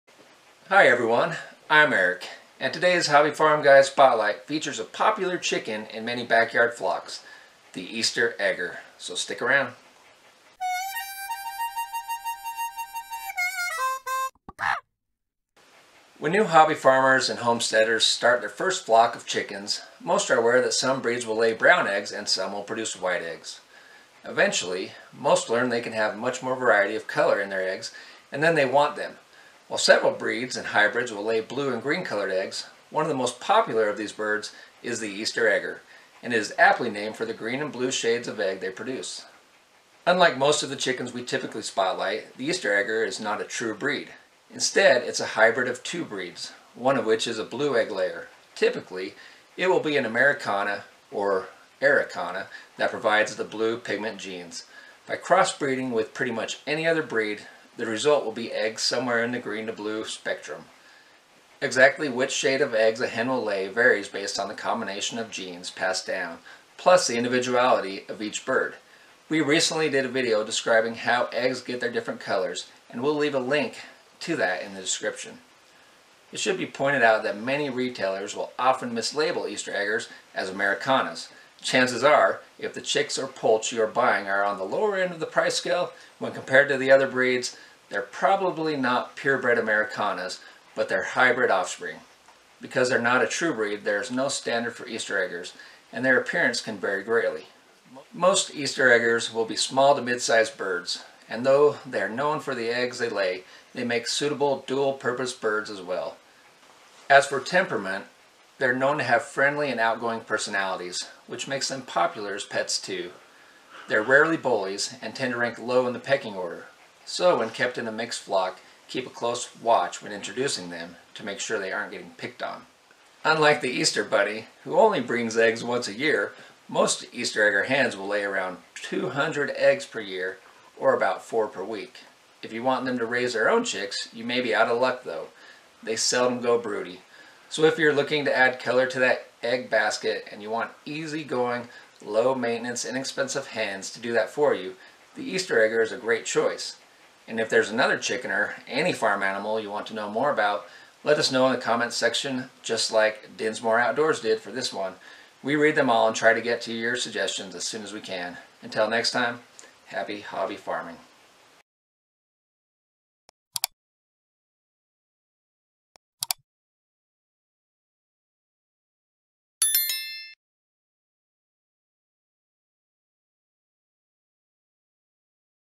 HFG Farm Animal Spotlight: Easter Egger Chickens
If you are looking for a chicken that will brighten up the egg basket with a little color, the Easter Egger might be for you. Eric puts the spotlight on this popular hybrid.

Link to "Colored Eggs and The Chicken Breeds That Lay Them" -